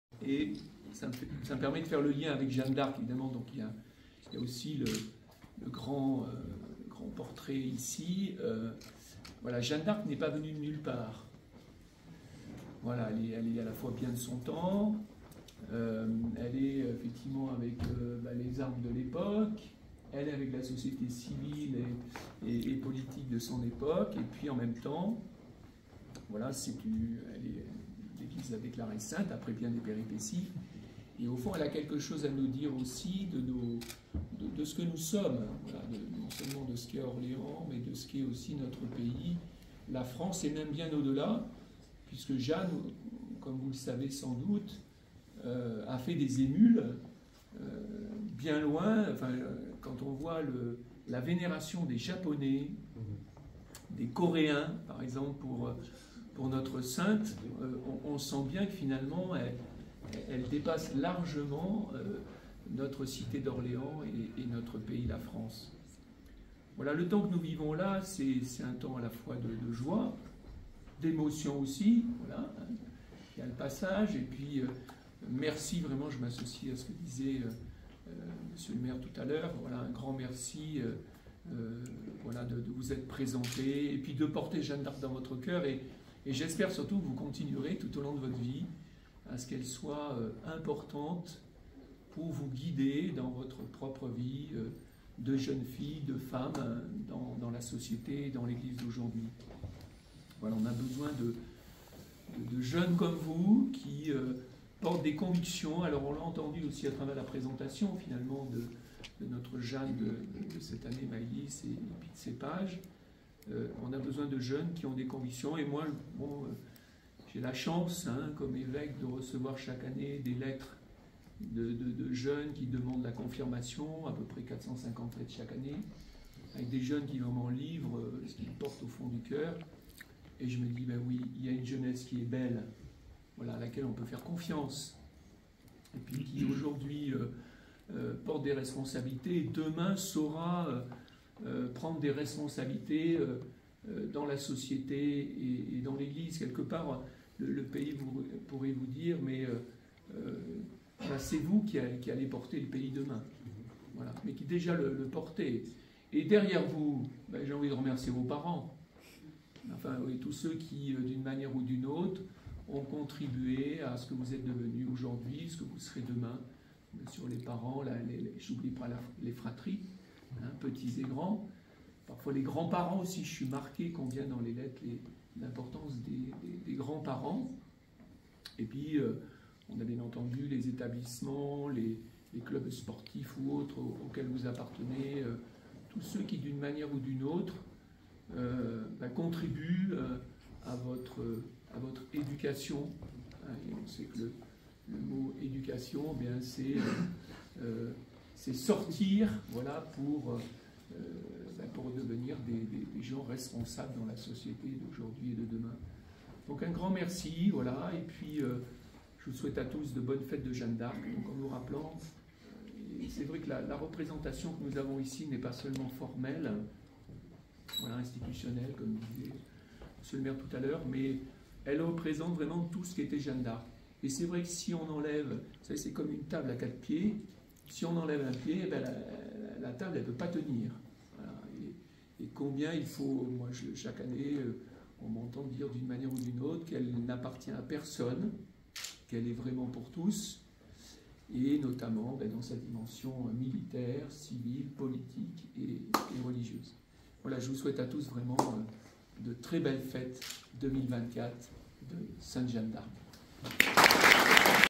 Prise de parole de Monseigneur BLAQUART Evêque d'Orléans
🛡  Le lundi  12 février, la Ville d'Orléans  et l’ association Orléans Jeanne d'Arc  ont présenté officiellement la jeune fille qui figurera Jeanne d’Arc lors des 595e Fêtes de Jeanne d'Arc d'Orléans
🗣 Prise de parole de Monseigneur BLAQUART Evêque d'Orléans,  juste avant que ne soit dévoilée l'affiche officielle des Fêtes de Jeanne d'Arc d'Orléans organisées du 29 avril au 8 mai 2024.